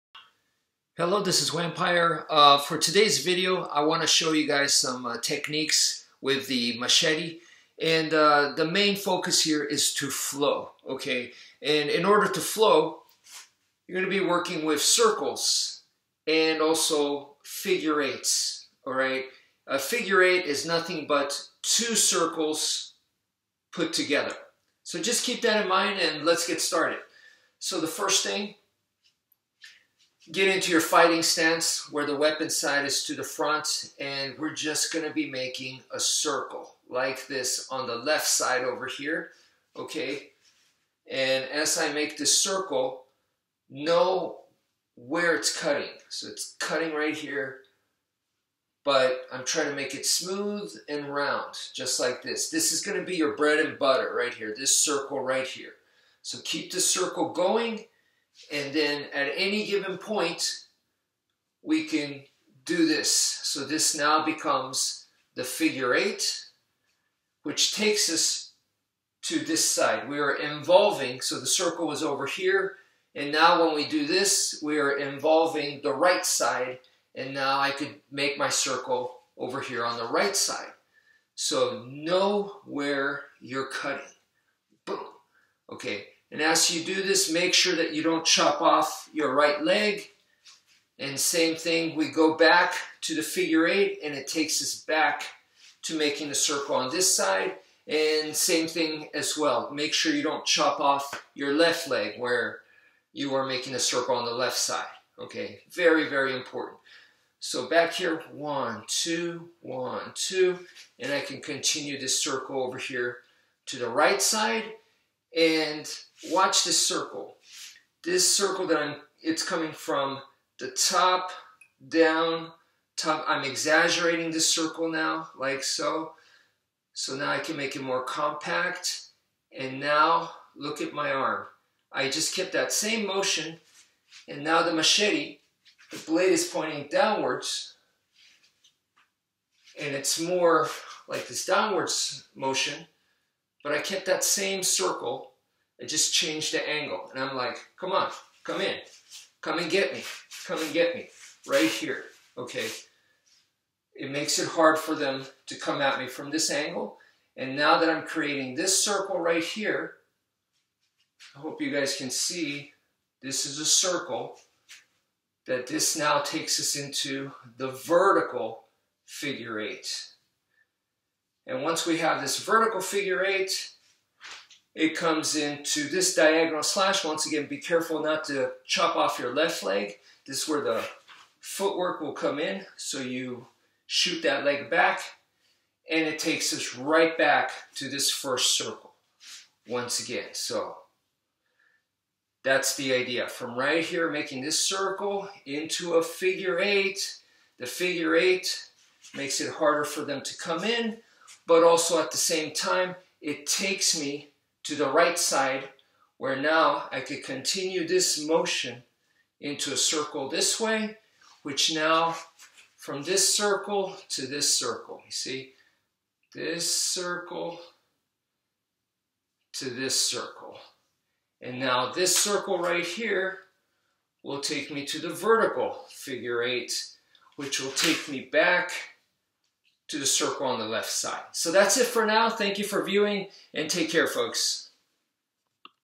Machete Flow techniques Escrima Arnis Kali
Machete flow techniques can be practiced with a stick so please do not use a real machete and use a stick instead.